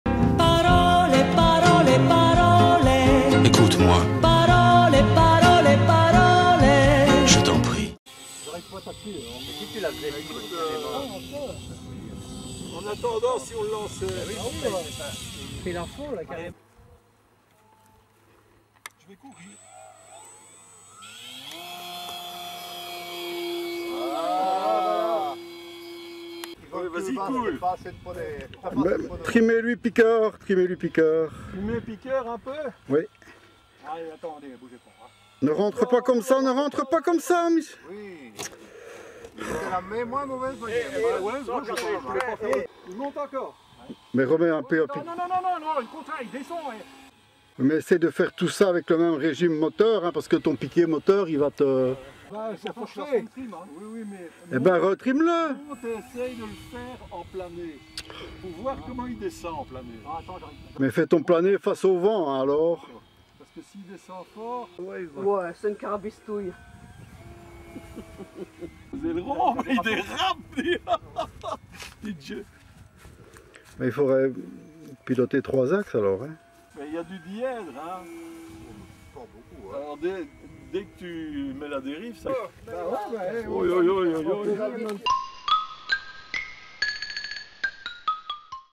avion imprimante 3 d movie
Vol du premier avion imprimé 3D au club par Michel